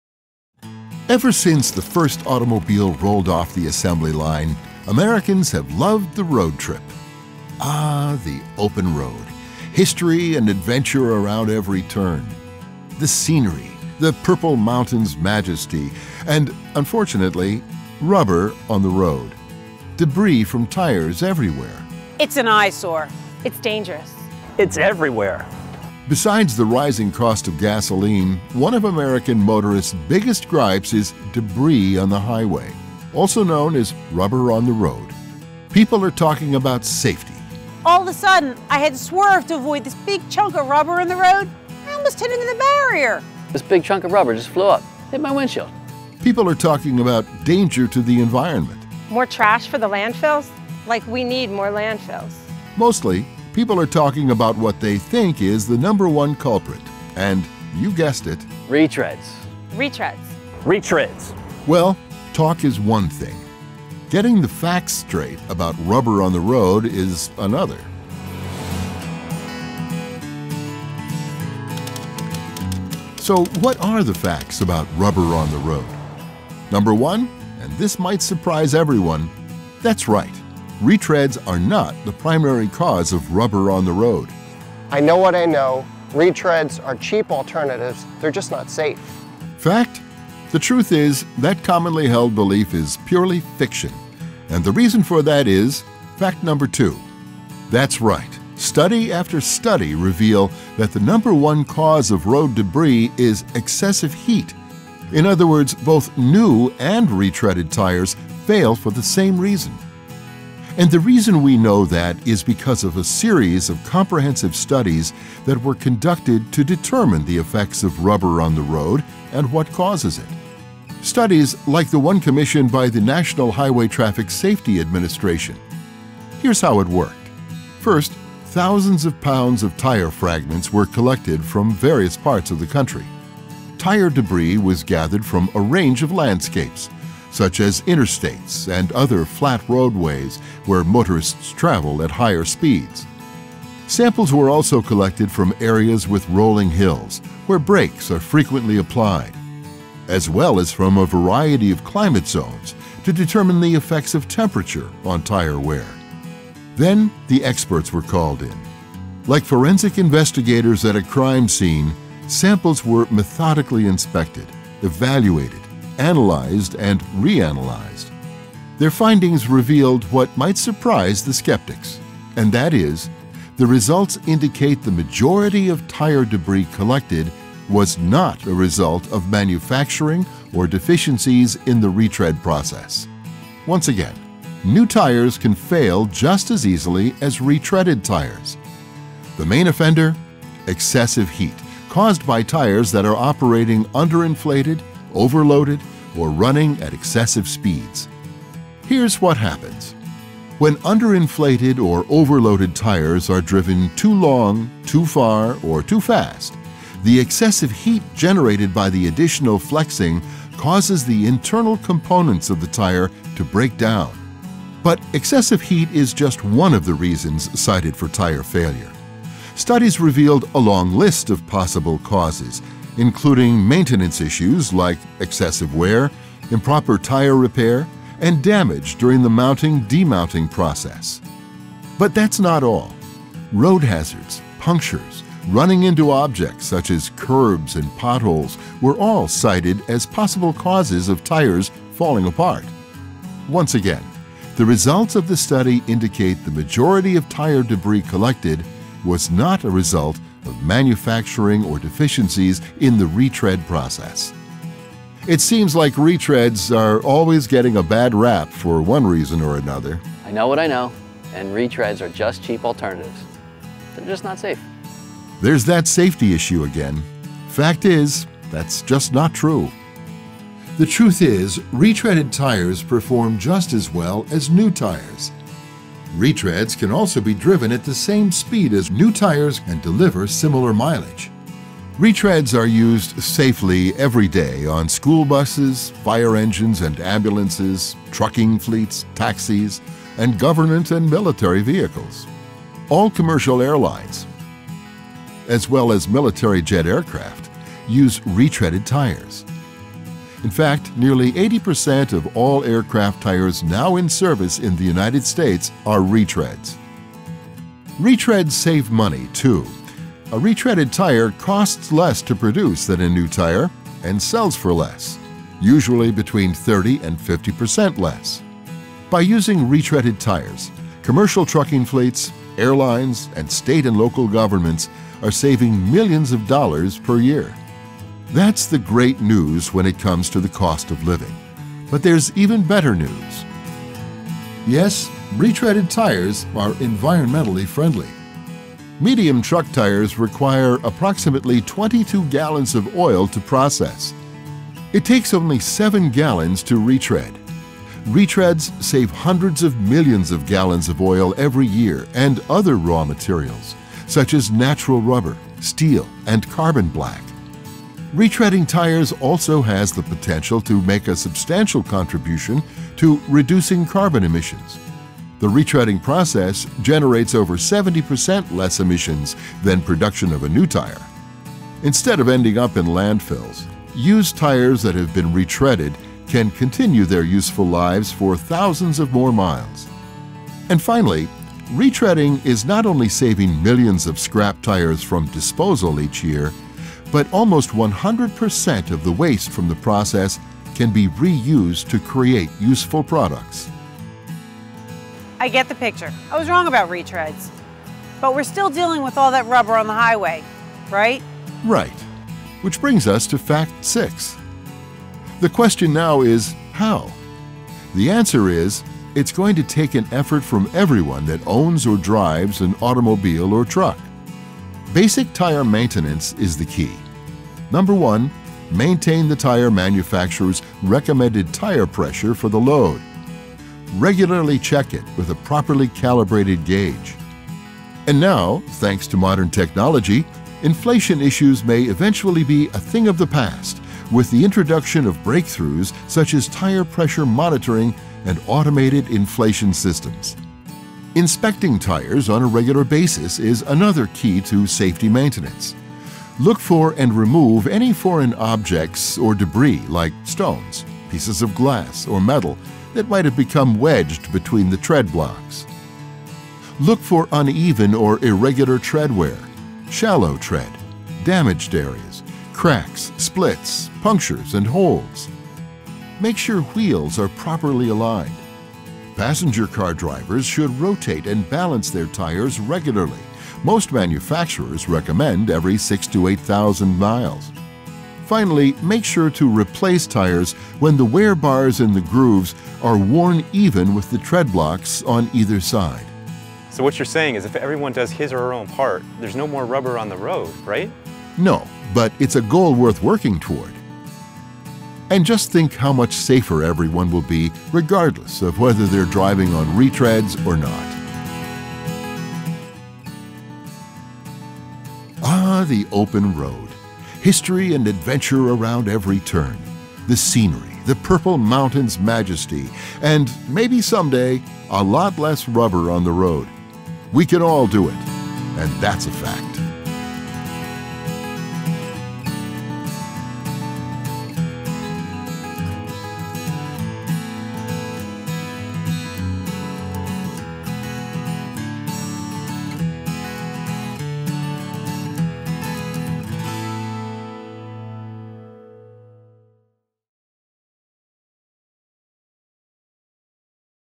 True Causes of Rubber on the Road
Many people blame retreaded tires for the rubber on our roads, but this persistent myth is false. Learn more about the true causes of rubber on our roads and what you can do to help.